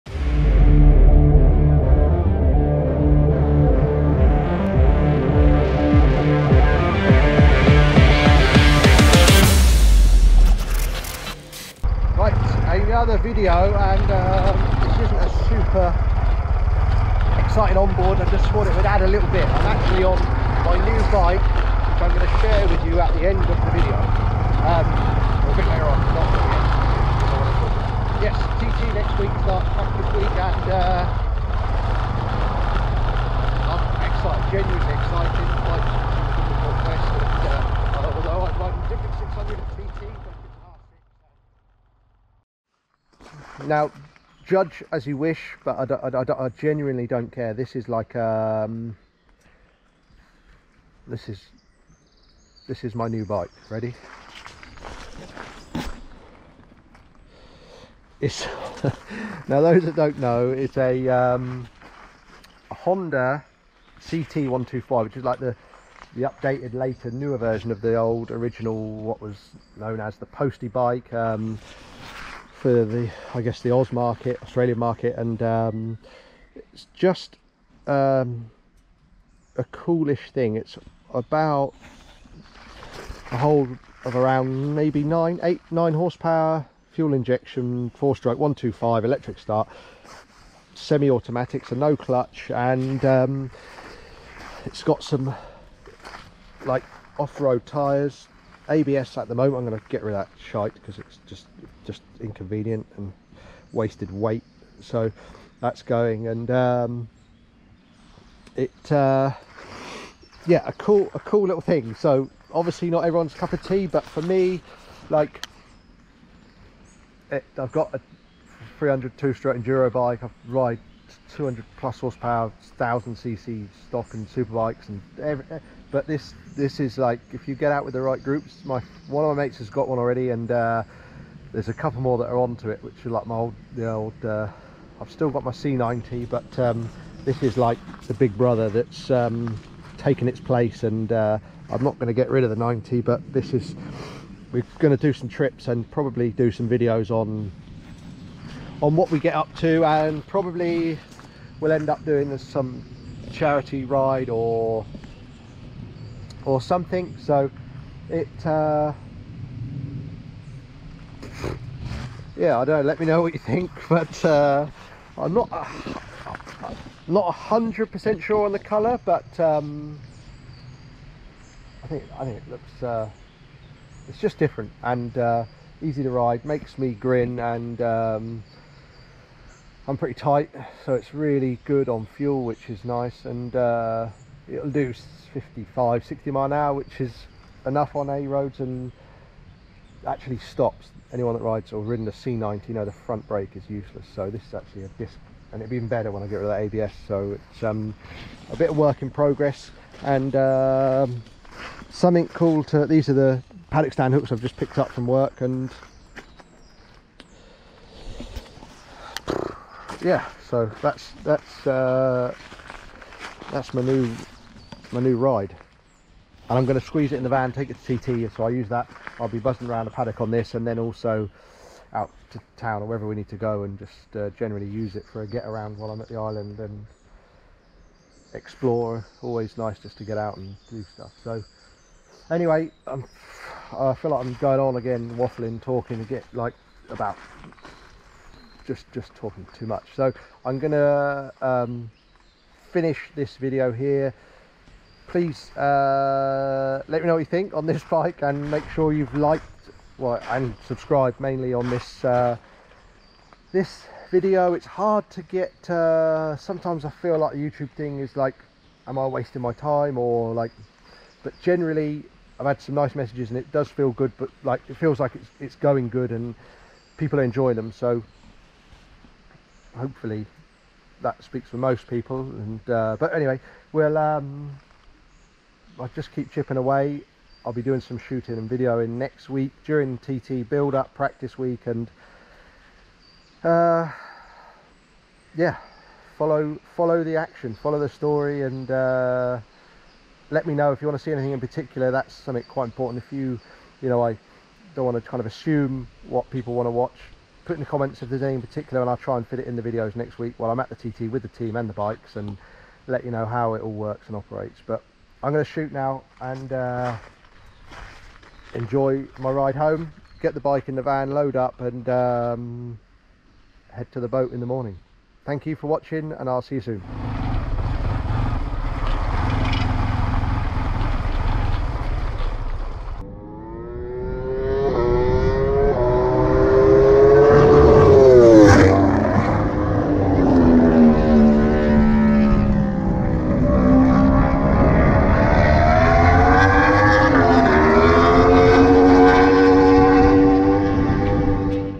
James' New Bike Reveal... You Won't Believe What He's bought!
A bit different this week. The day before heading to TT decided to take my new bike out on the scenic route to collect some parts for the TT bike and wanted to share the ride and talk post-North West 200 and pre-TT.

Unfortunately audio of talking whilst riding did not work!

But basically, bikes felt good at NW200, I genuinely enjoyed riding them. Didn’t end well with a little high side but beat me up a bit landing back on the bike. Nothing was broken luckily!

TT starts next, and I really can't wait to return to that course!

Will go into more detail in the next video and get the audio sorted!!  (Sorry)

James